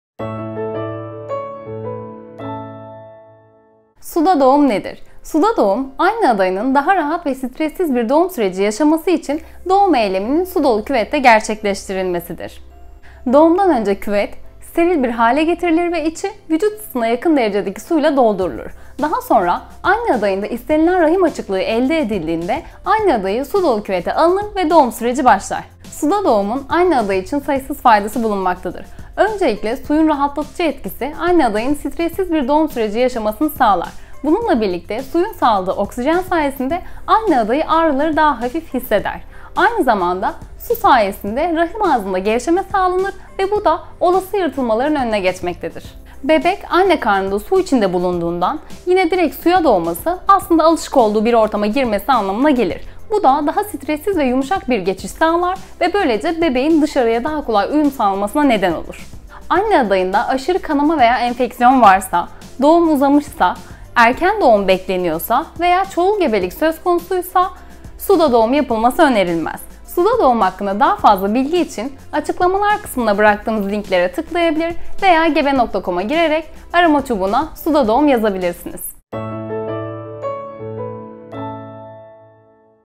Suda Doğum Nedir? Suda Doğumun Faydaları Nelerdir?
Suda doğum nedir? Suda doğumun anne ve bebek için faydaları nelerdir?  Hangi durumlarda suda doğum yapılmaz?

Suda doğum hakkında daha fazla bilgi için aşağıdaki linklere tıklayabilirsiniz.

Suda Doğum Nedir, Faydaları Nelerdir?

Suda Doğum Bebek İçin Zararlı mıdır?

Bizi İnstagram hesabımızdan da takip edebilirsiniz.